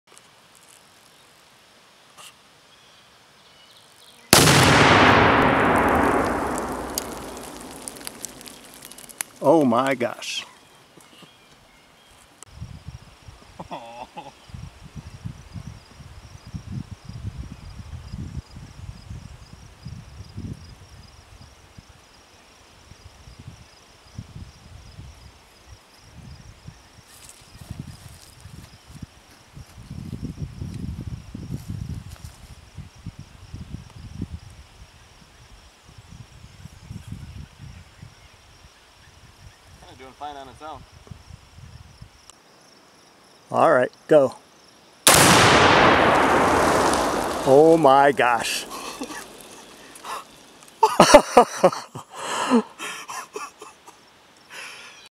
Tannerite Exploding Targets
Testing the 1 lb. tannerite exploding targets.  The first one is inside a watermelon.  The second was placed on a short 1x4 board and floated on a pond.  Both are shot with an AR (.223).